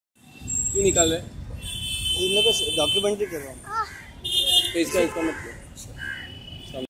Ear Cleaners Removing paper from a child's ears
this child had the habit of shoving pieces of paper in his ears ,.. so the earcleaner is removing it ,,a painful task..